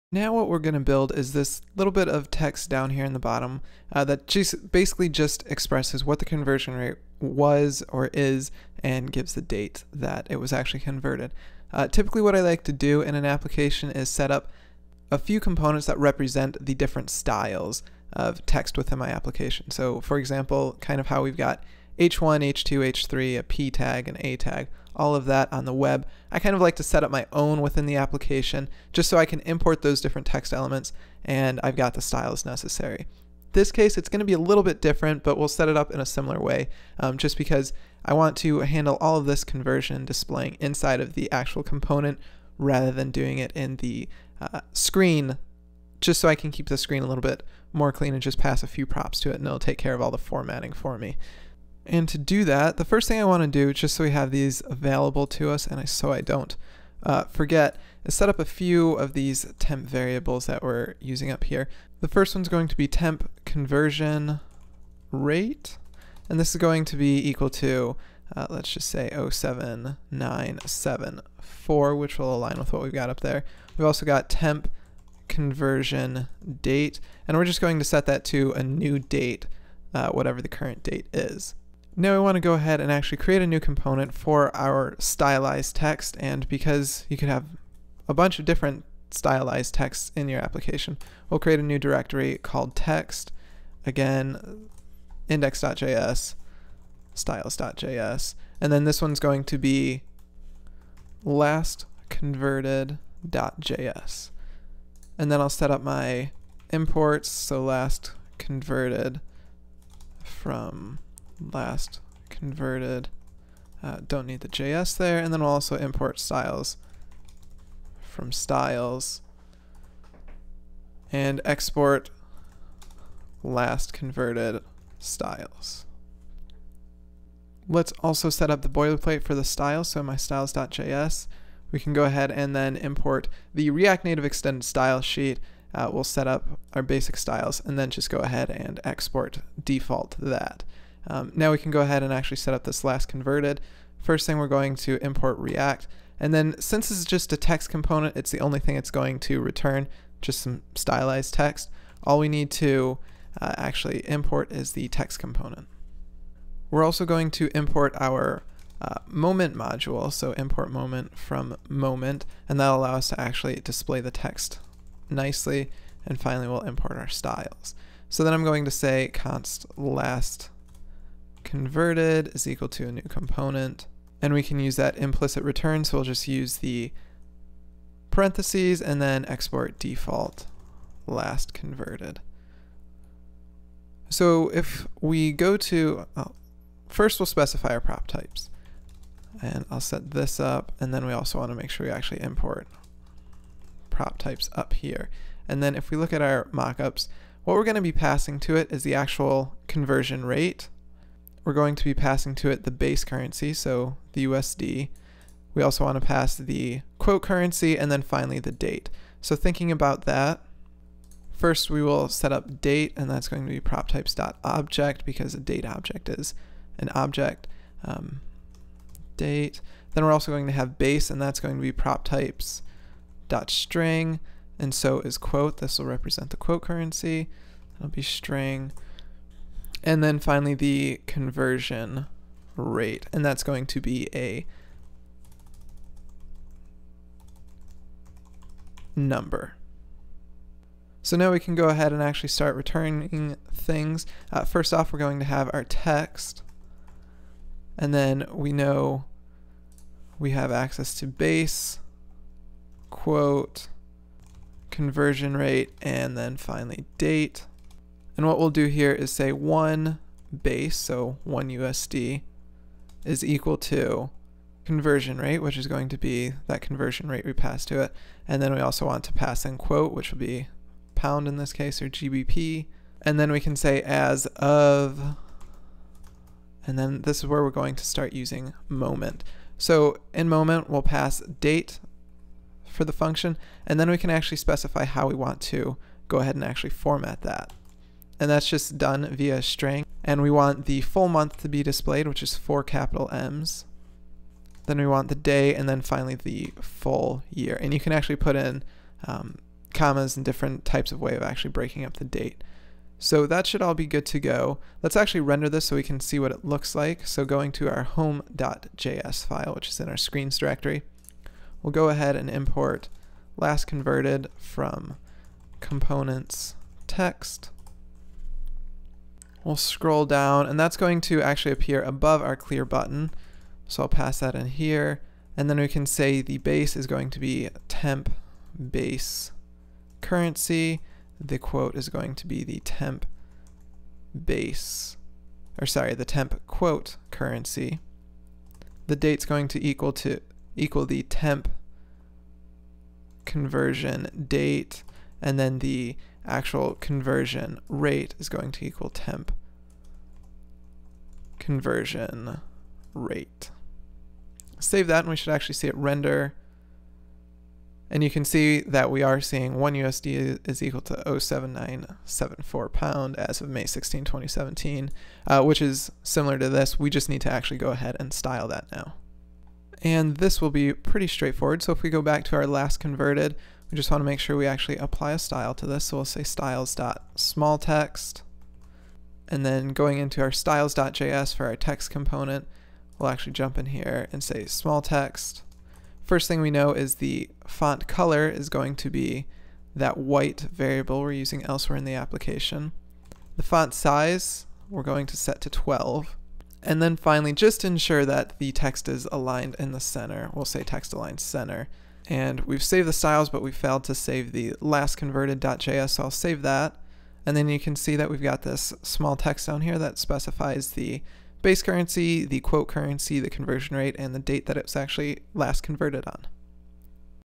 Home Screen Styled Text - Build a React Native Currency Converter
Learn how to build the interface of a currency converter application in React Native! This series of videos is only one module of seven from my free course. If you like this set of videos and want to continue be sure to visit reactnativebasics.com!